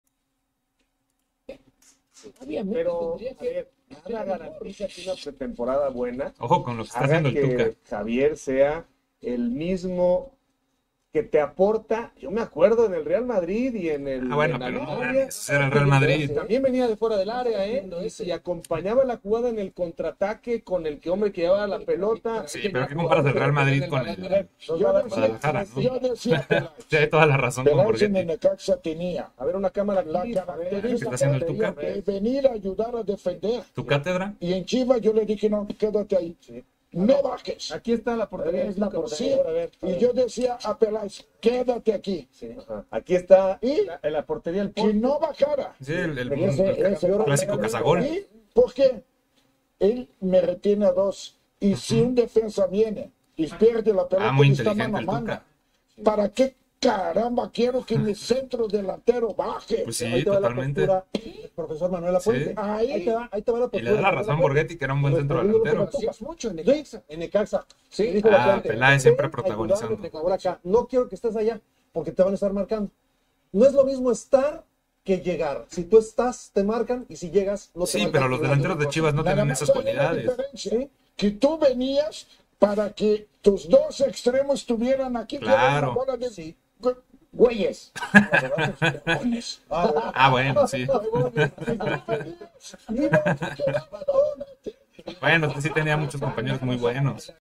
El TUCA da clase en vivo.
El TUCA Ferreti explica el porqué un centro delantero debe quedarse en el área.